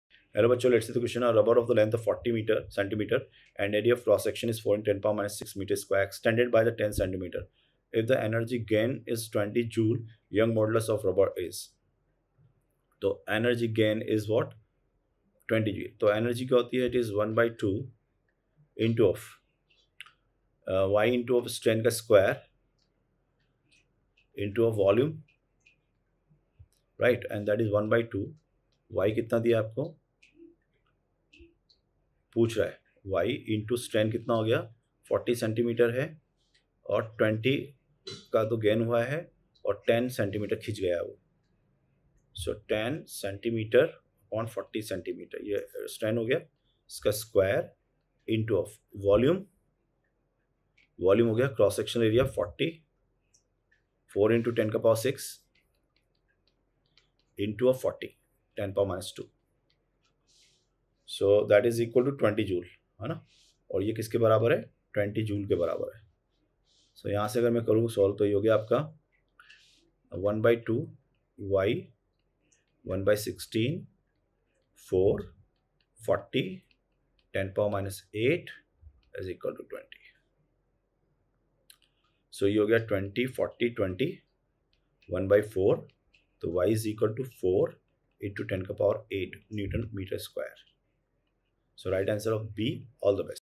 A rubber cord of length 40 cm and area of cross section 4 × 10^-6 m^2 is extended by 10 cm. If ...
A rubber cord of length 40  cm and area of cross section 4 × 10^-6 m^2 is extended by 10  cm. If the energy gained is 20 joule, Young's modulus of rubber is
a. 10^8Nm^-2
b. 2 × 10^8Nm^-2
b. 4 × 10^8Nm^-2
d. 1.5 × 10^6Nm^-2